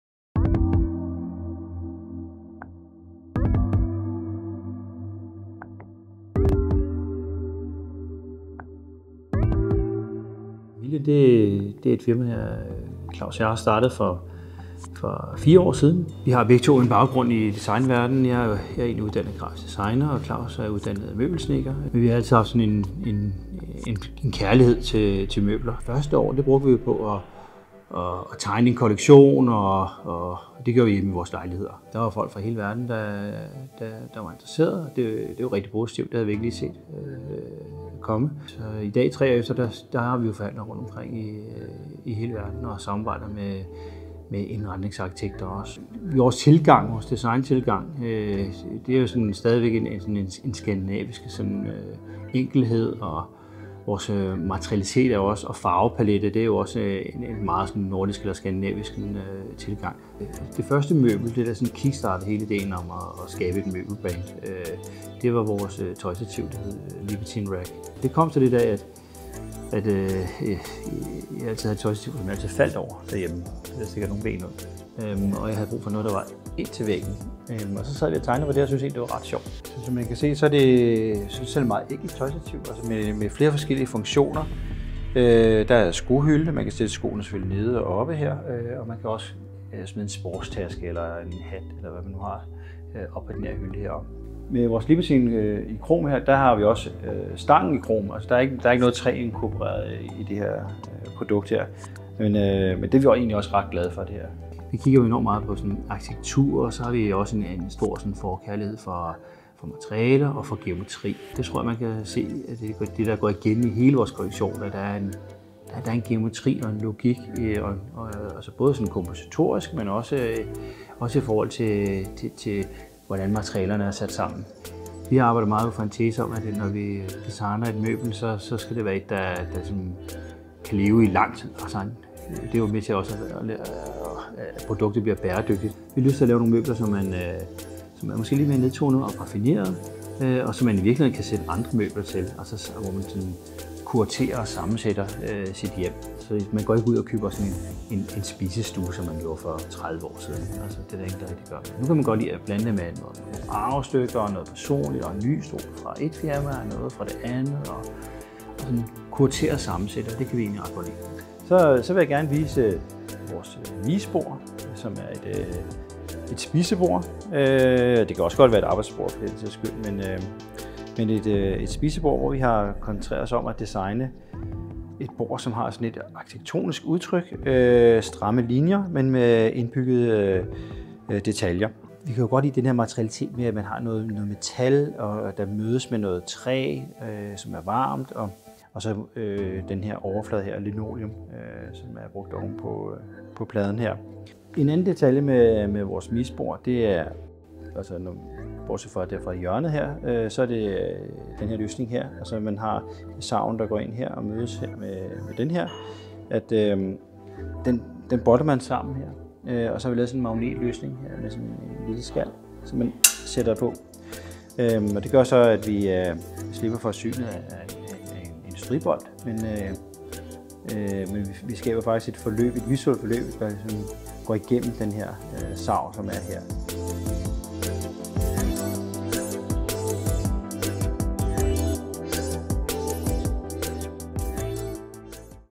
MILLION CPH
Mød det danske brand, der laver bæredygtige luksusmøbler med praktik og design i perfekt forening. Moodings har talt med Jens, der er en af designerne bag det populære brand, om alt fra det første MILLION produkt til livsstilstrends og bæredygtigt design, der ligner en million.

Hvis du er blevet inspireret kan du selvfølgelig shoppe hele Million's smukke kollektion på